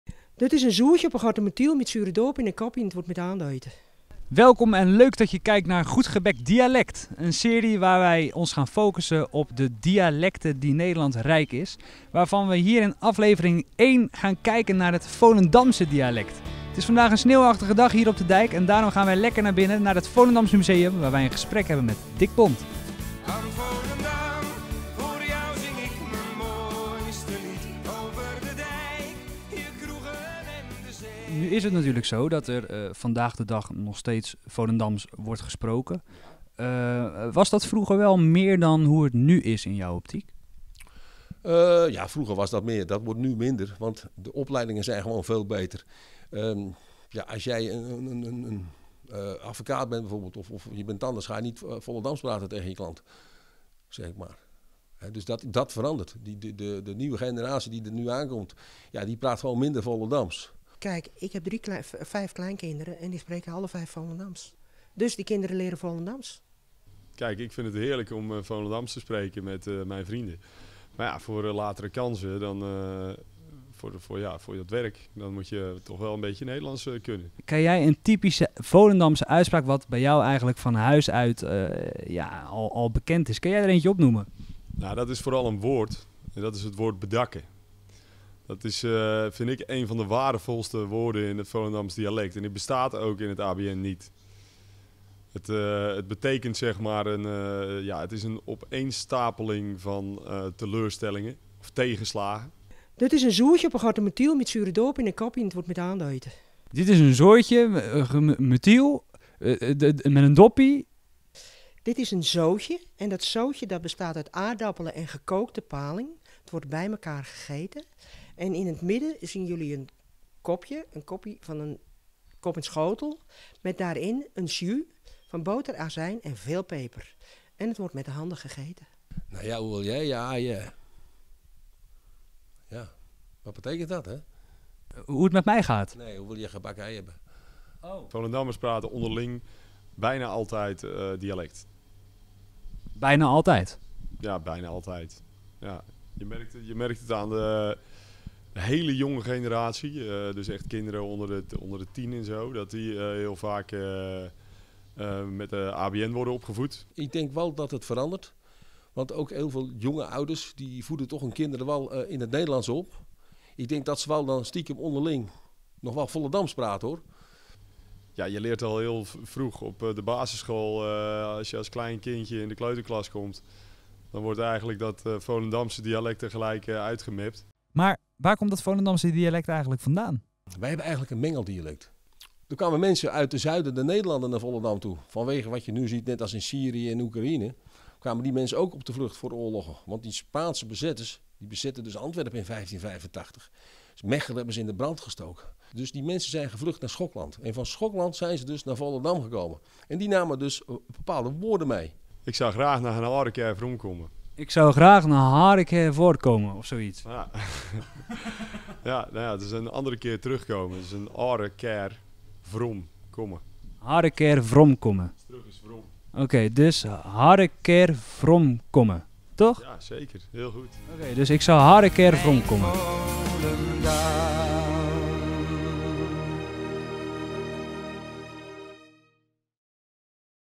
HOE POPULAIR IS DE VOLENDAMSE TAAL NOG? | Goedgebekt Dialect - Volendam - Aflevering 1
Dialecten maken een groot deel uit van de lokale culturen in Nederland. In onze driedelige serie ‘Goedgebekt Dialect’ doorkruisen we ons land om kennis te maken met verschillende dialecten en onderzoeken we hoe de plaatselijke taal leeft.

Camera: Pepijn van Oirschot